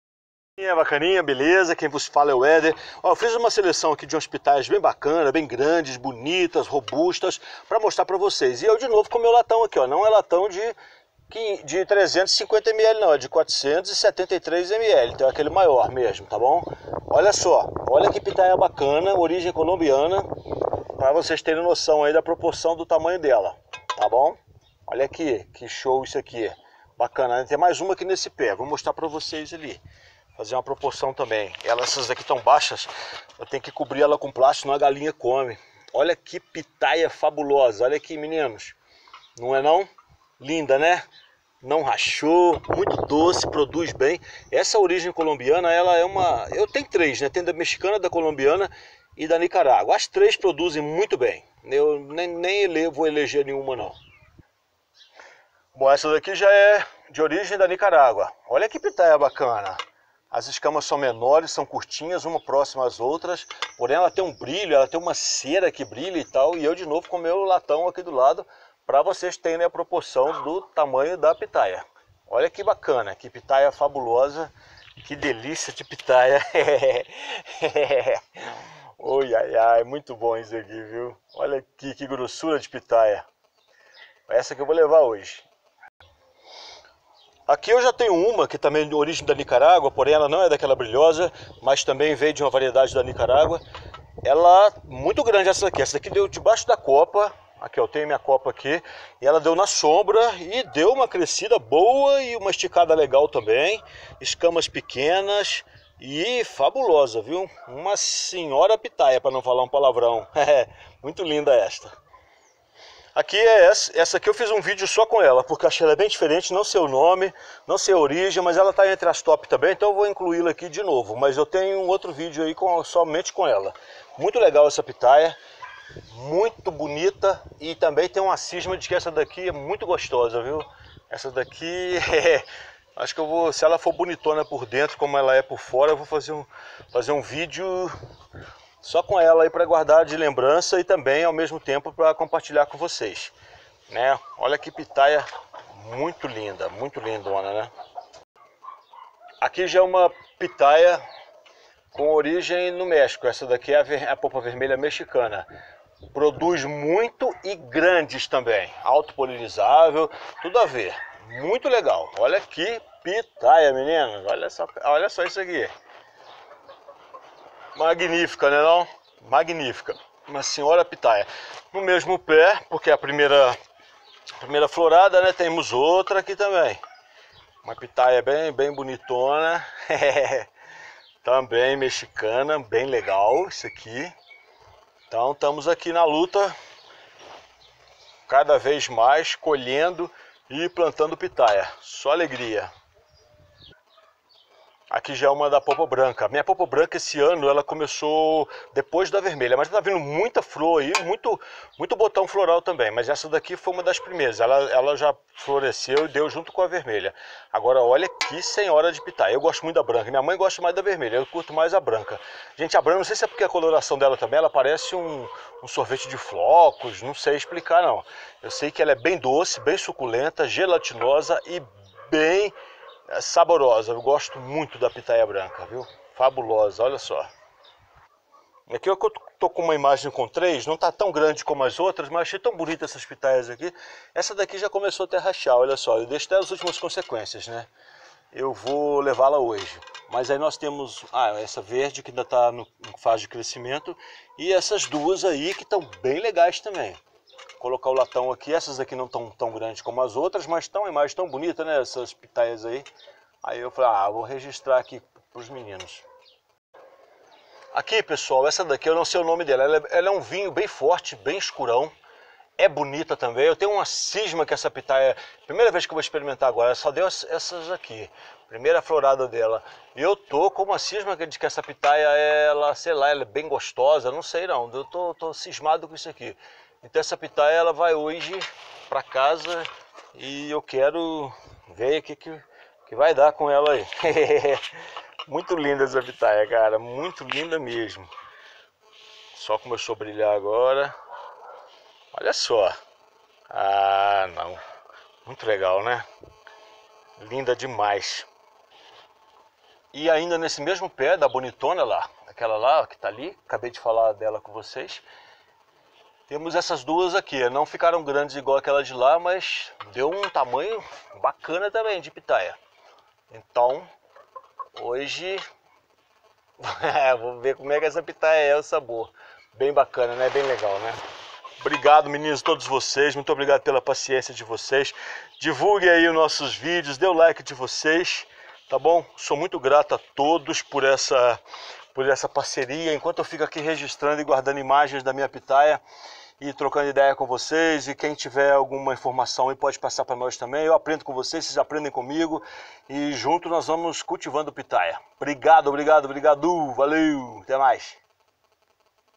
As minhas Pitayas TOP's (de 670 a 830 gs)
Fiz uma seleção das maiores que produzi - a maior tem 830 gramas.